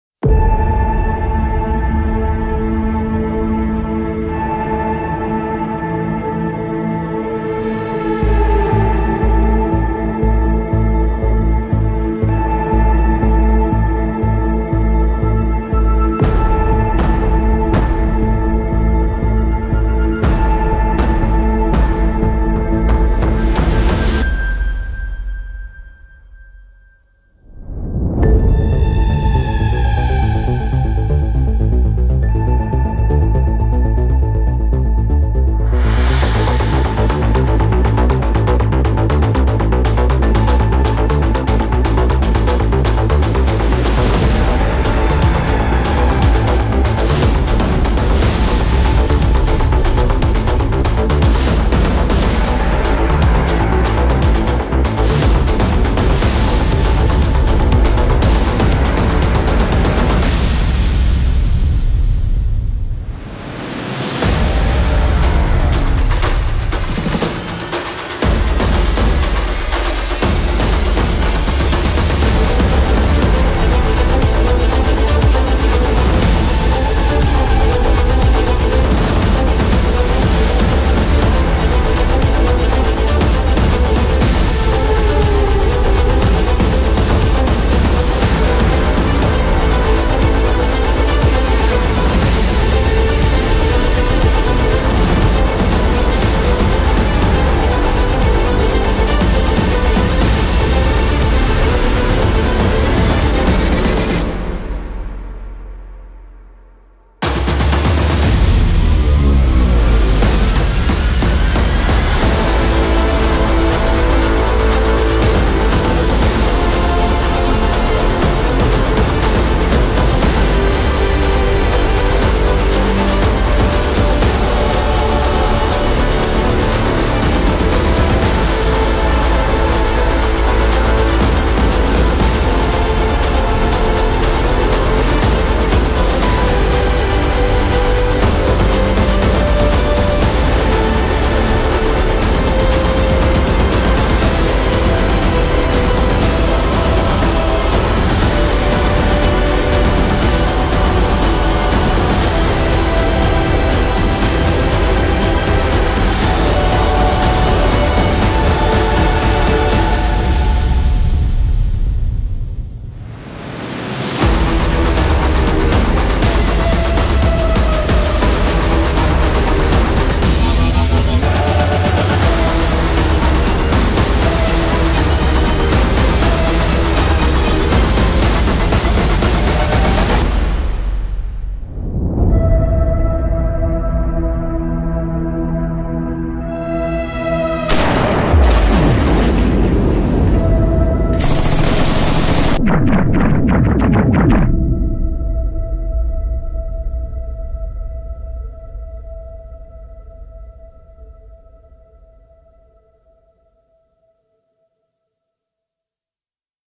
[VFX TIMELAPSE] Underwater Bullet Trails
Timelapse of the compositing work using the Underwater Bullet Trails stock assets, available for download